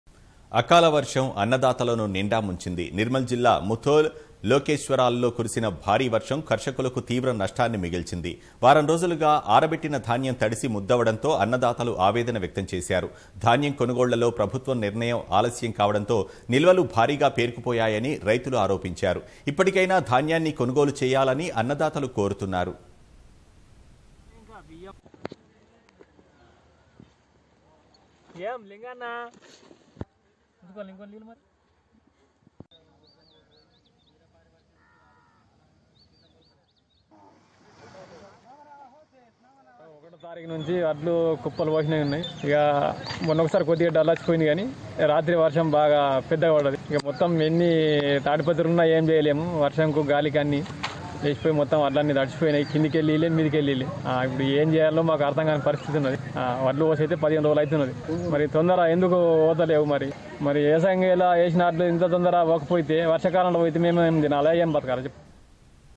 Crop Damaged in Huge Amount | Due to Unseasoned Rains | at Nirmal, Muthol & Lokeswaram
అకాల వర్షం అన్నదాతలను నిండాముంచింది. నిర్మల్ జిల్లా ముధోల్ , లోకేశ్వరాల్లో కురిసిన భారీవర్షం కర్షకులకు తీవ్ర నష్టాన్ని మిగిల్చింది. వారం రోజులుగా అరబెట్టిన ధాన్యం తడిసి ముద్దవడంతో అన్నదాతలు ఆవేదన వ్యక్తం చేశారు. ధాన్యం కొనుగోళ్లలో ప్రభుత్వ నిర్ణయం ఆలస్యం కావడంతో నిల్వలు భారీగా పేరుకుపోయాయని రైతులు ఆరోపించారు. ఇప్పటికైనా ధాన్యాన్ని కొనుగోలు చేయాలని అన్నదాతలు కోరుతున్నారు